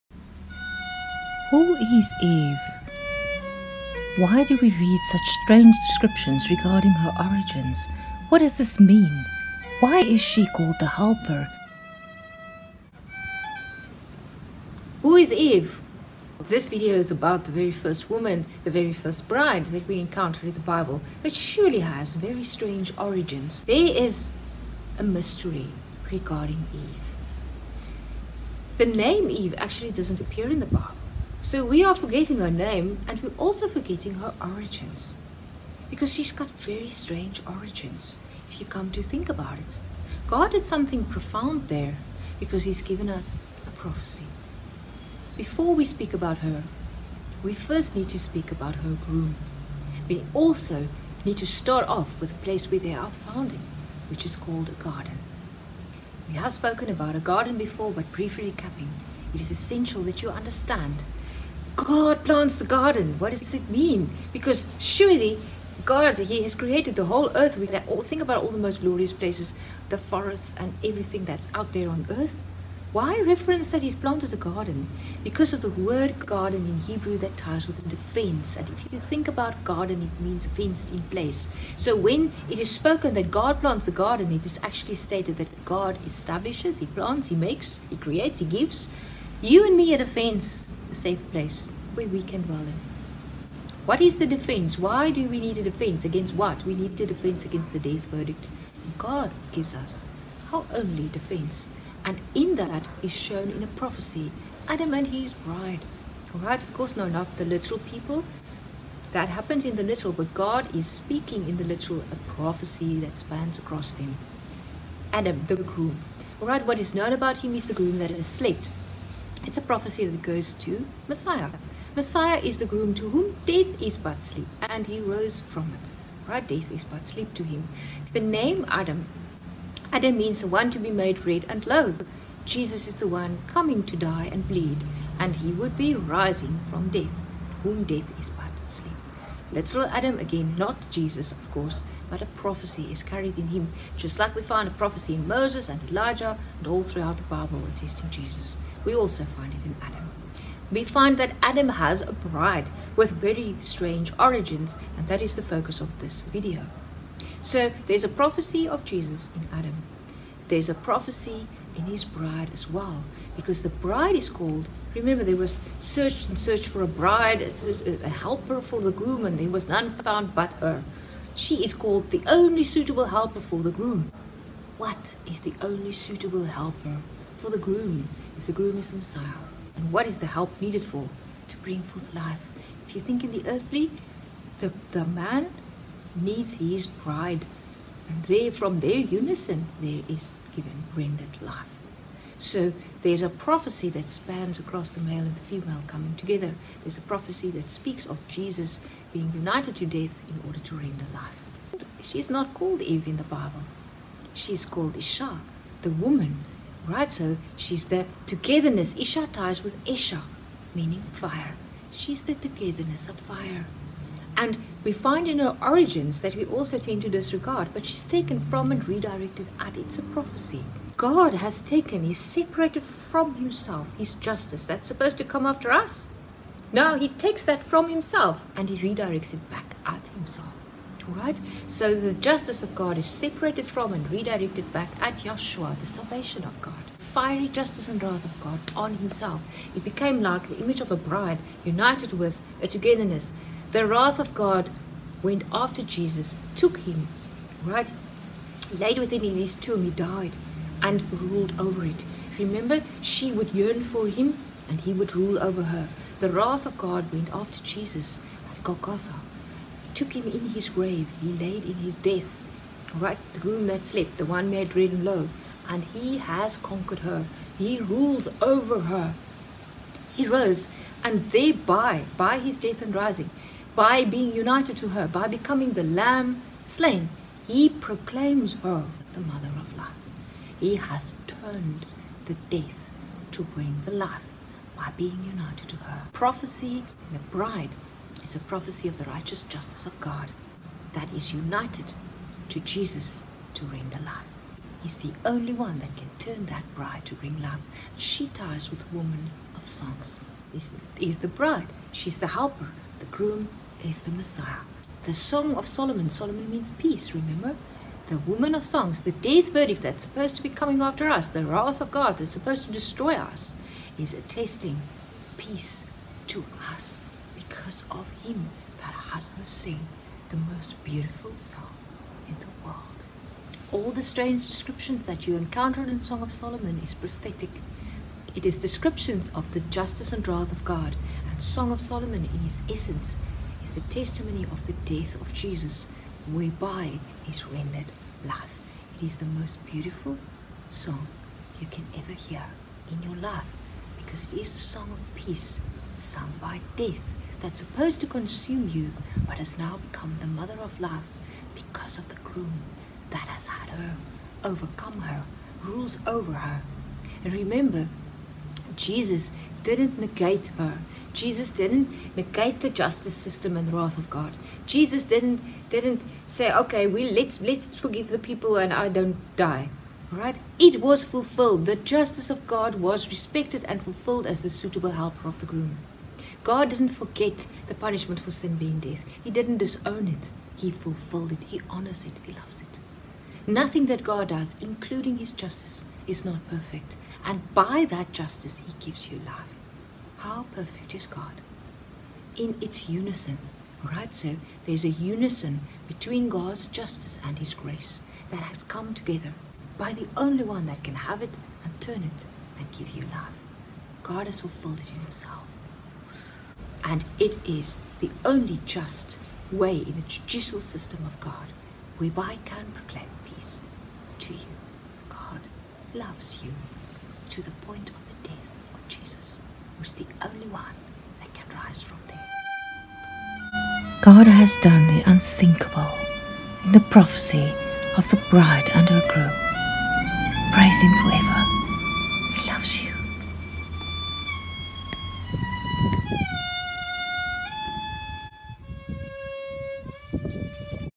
MYSTERY of EVE! Amazing.
Mystery of Eve! Astounding what God has done! God placed Prophesy in the first Groom and Bride! It ties to Song of Solomon! This is awesome! Praise God!